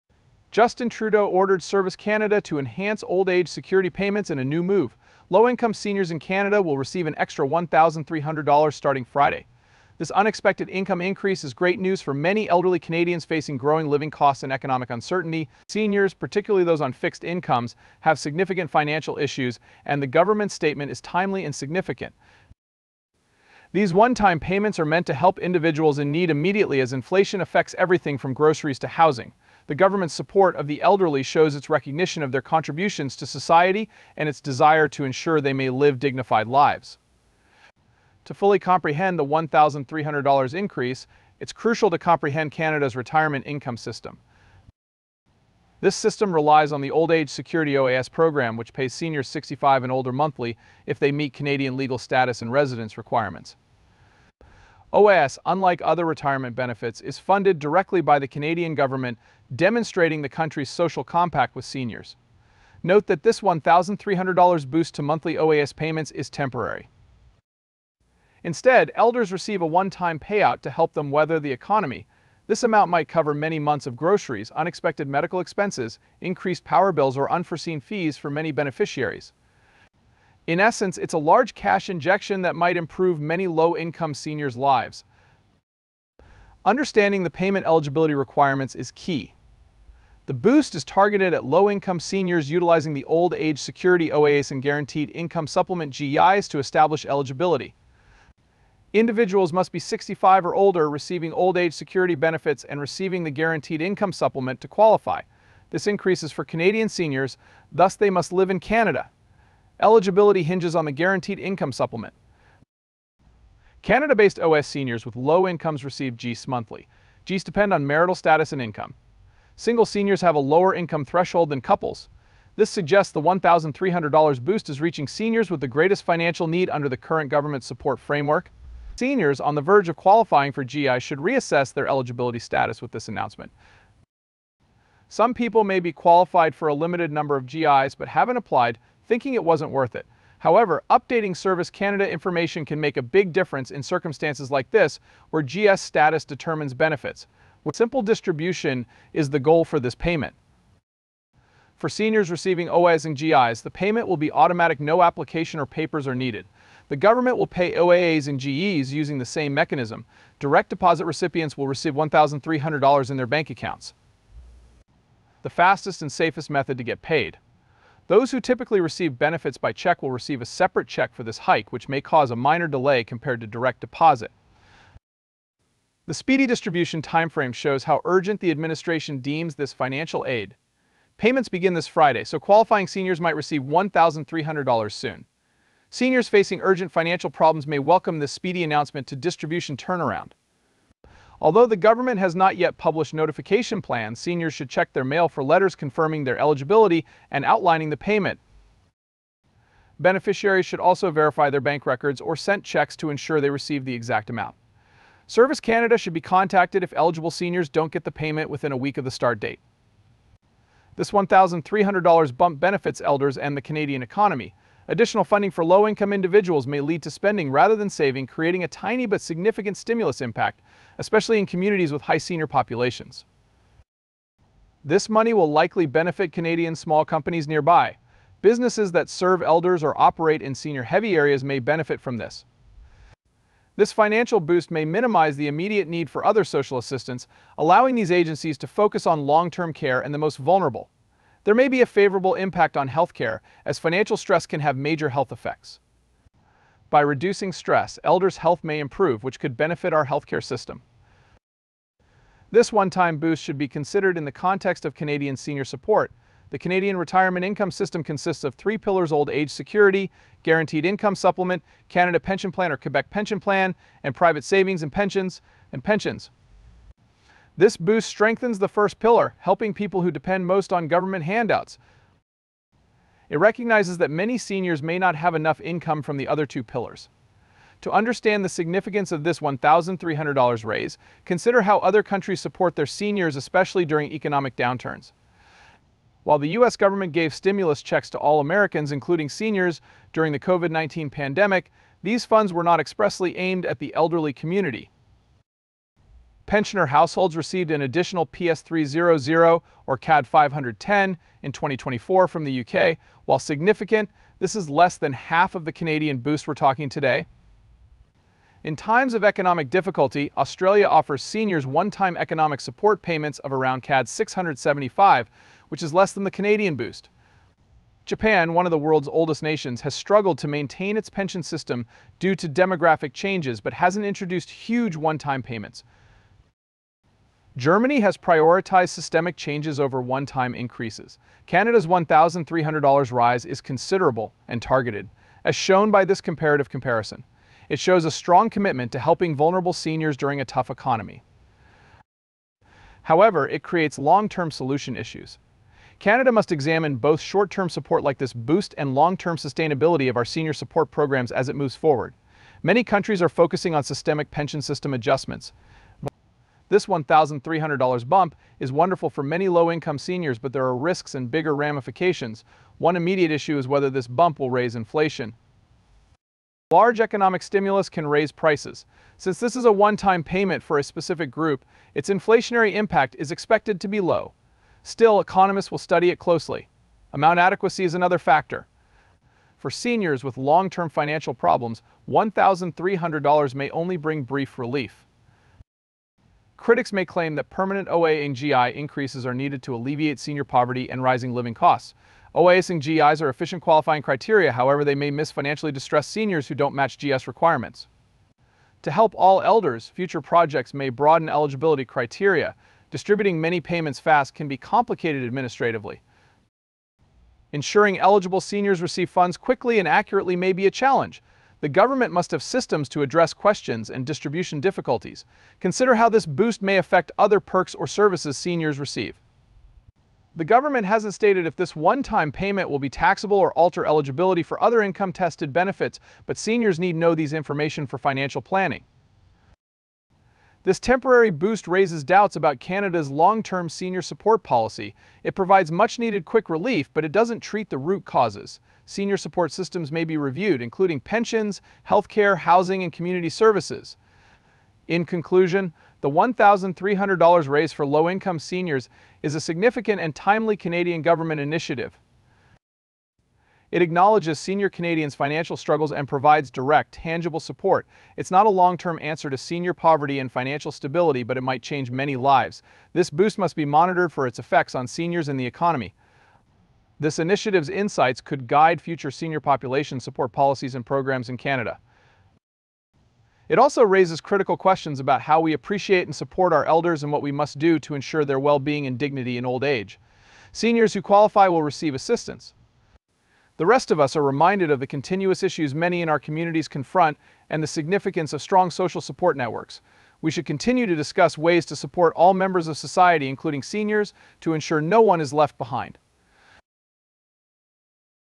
Trudeau Instructed Service Canada to increase OAS by $1300 for low-income this Friday.
Prime Minister Justin Trudeau has directed Service Canada to increase the Old Age Security (OAS) payout by $1300 on Friday, significantly benefiting low-income seniors. 🌟 Our senior population requires essential financial support in retirement, and this substantial enhancement aims to enhance their quality of life. 🌼